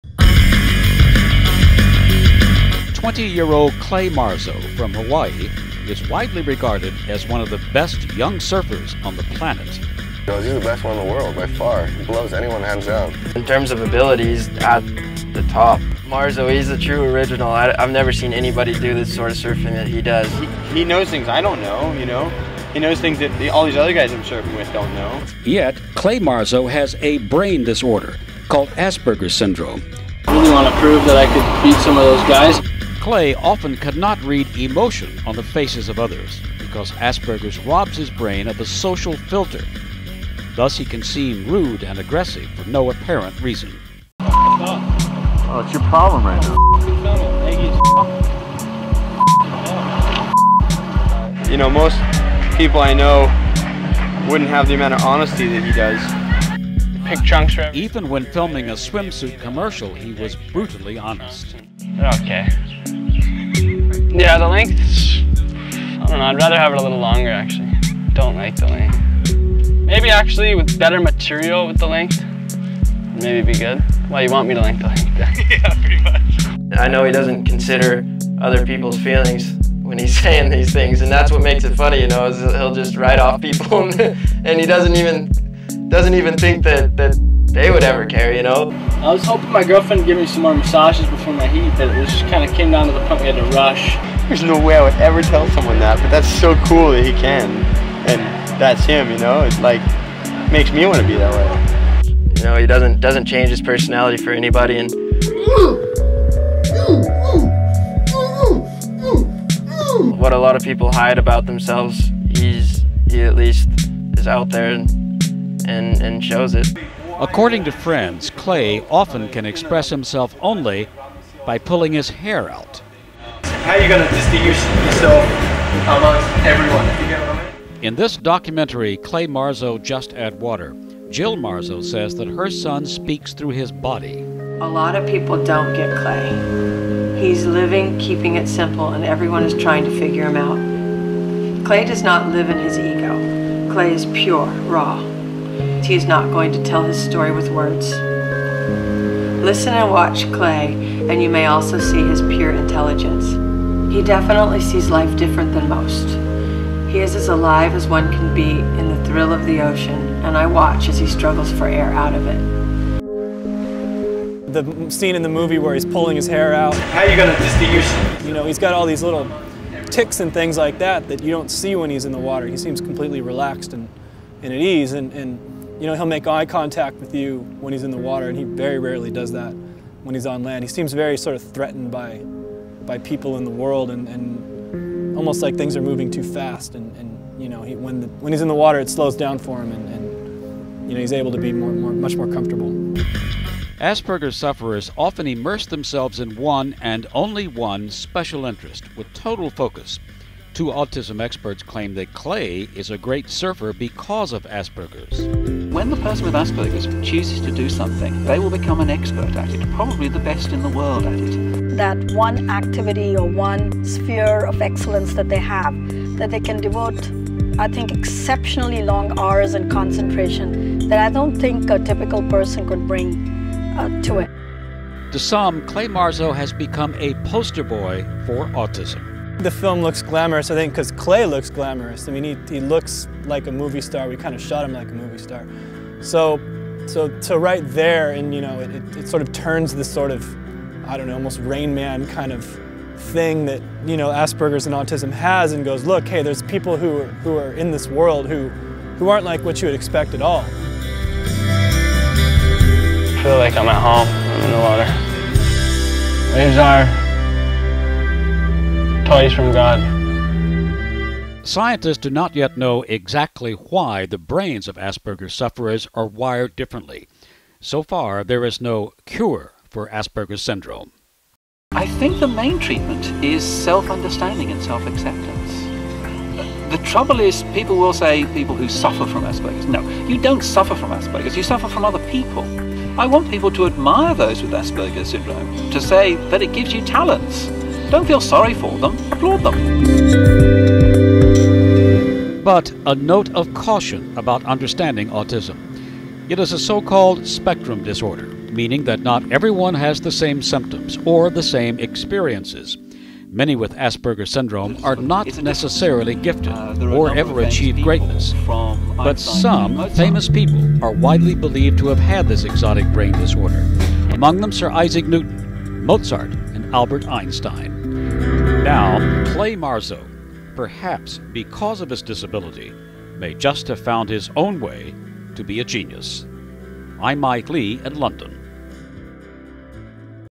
Clay Marzo- Surfer with Aspergers - Seems to Work For Him
Clay Marzo is widely considered one of the best young surfers on the planet.
But, say some experts, is greatness in the water may in part be due to his inability to function normally on land.  Clay has Aspergers Syndrome, a form of Autism, and that may give him a huge edge over oherr top surfers.  Watch and judge for yourself.